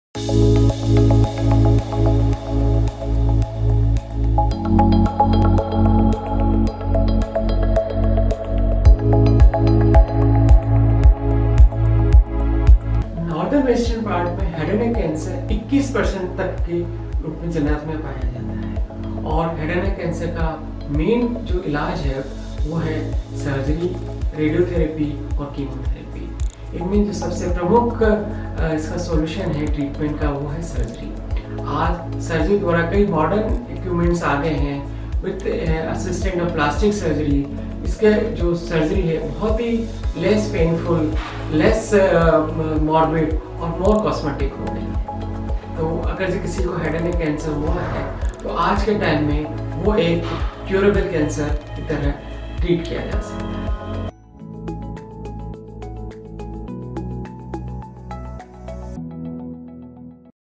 Treatment of Head and Neck Cancer | सिर और गर्दन के कैंसर का उपचार - Dr. Naresh Ledwani
Radiation therapy, surgery, and chemotherapy are the three main treatments for head and neck cancer. Either radiation therapy, surgery, or both combined are typically used as the primary treatments with the goal of killing or removing the cancer. Chemotherapy is often used as an additional, or adjuvant, treatment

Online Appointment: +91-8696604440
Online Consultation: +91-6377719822

Address: BMCHRC, Jawahar Lal Nehru Marg, Jaipur 302017